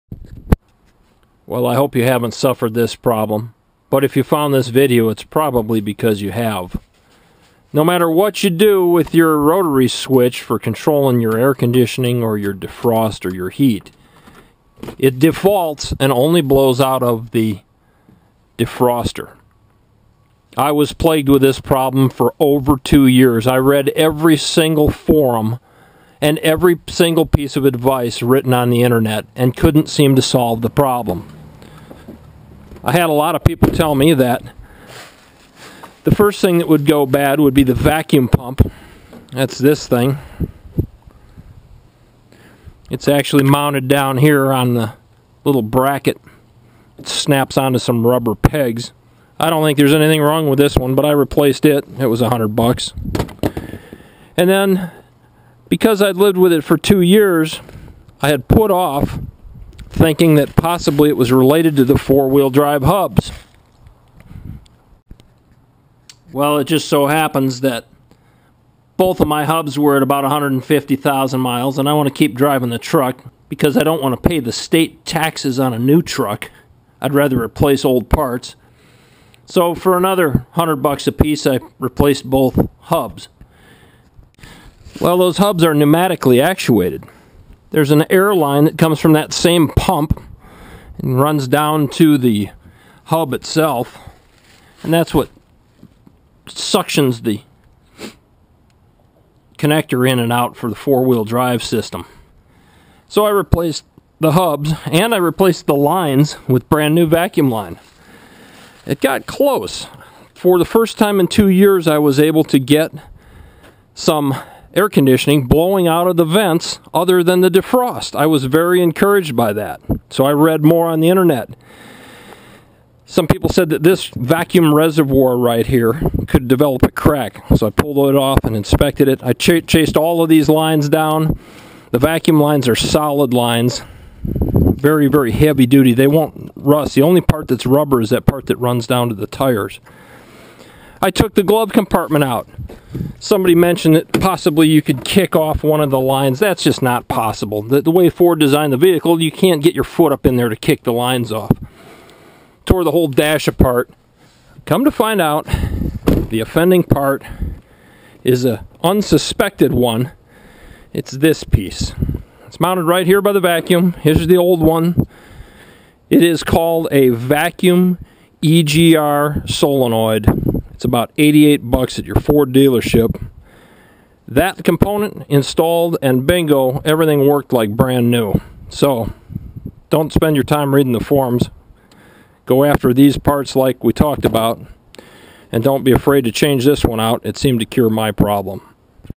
Ford F-250 stuck in defrost, problem solved
Air conditioning flow control, often fails on higher mile Ford trucks that use electric vacuum pumps that create suction.  The suction system not only controls air zone valves, but also 4X4 activation.  Typical vacuum leaks, like old lines are often the problem, but there is one last component that everyone participating on automotive help forums fails to mention when troubleshooting this trouble prone system, the electric vacuum solenoid valve.